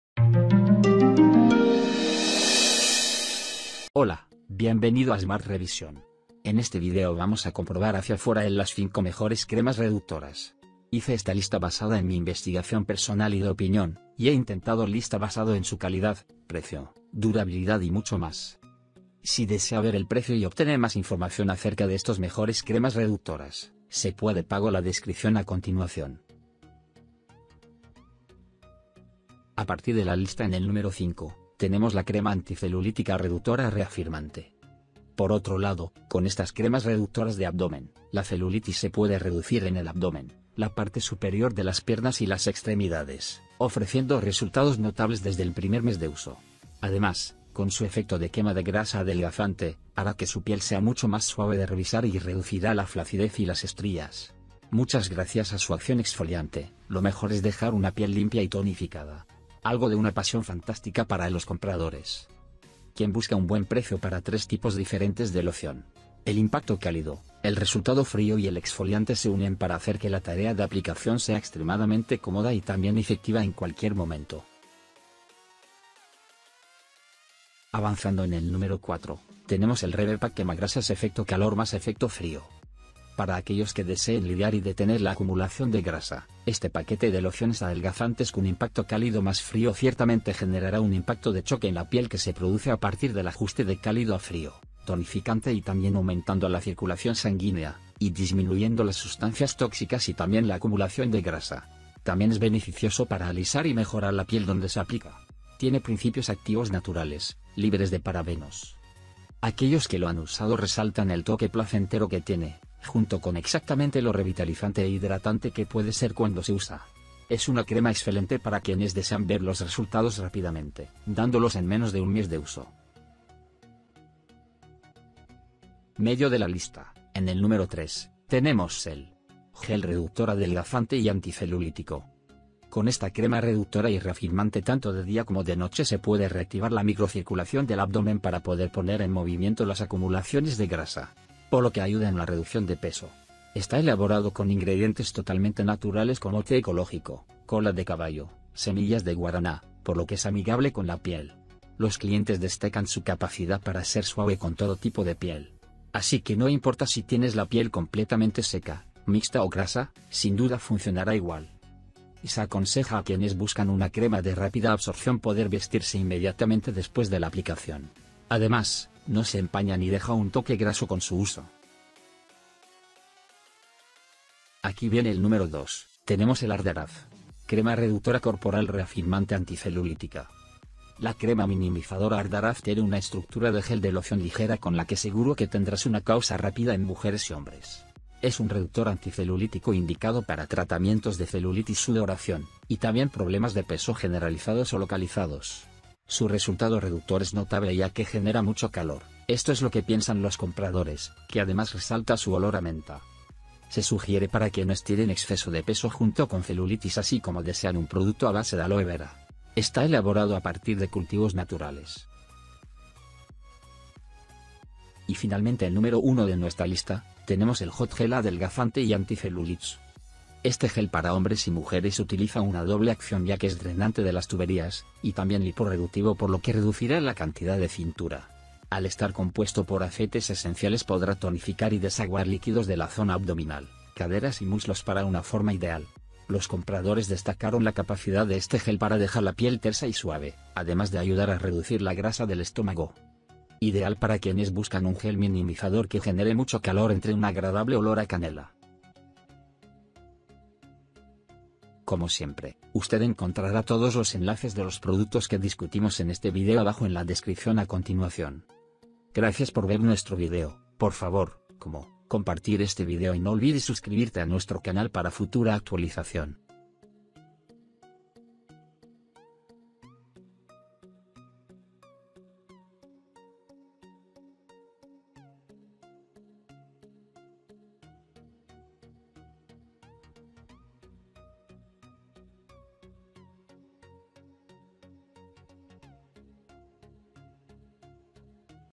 ✅ Mejores Cremas Reductoras 2022 - (Top 5)
Aquí está la lista de 5 Mejores Cremas Reductoras

1.Hot Gel Adelgazante y Anticelulits

2.ARDARAZ. Crema Reductora Corporal

3.Gel Reductor Adelgazante y Anticelulítico

4.REVE PACK Quemagrasas Efecto Calor

5.Crema Anticelulítica Reductora Reafirmante

Como socio de Amazon, gano de ventas calificadas.